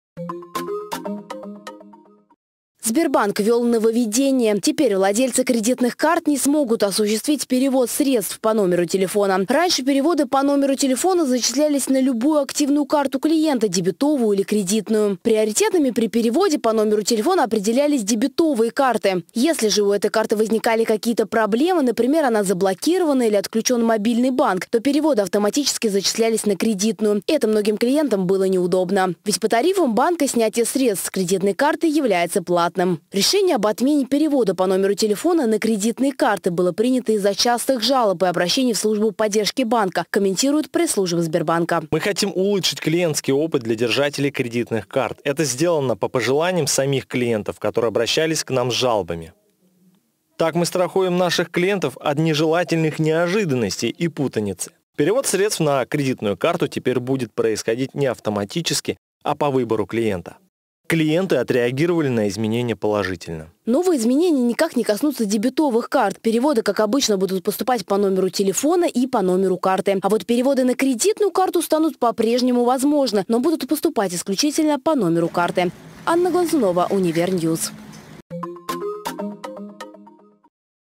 Сбербанк отменил перевод по номеру телефона
Сбербанк отменил перевод средств по номеру телефона. Нововведение не коснется дебетовых карт, это исключительно сделано для владельцев кредиток. Анна Глазунова разбиралась в ситуации.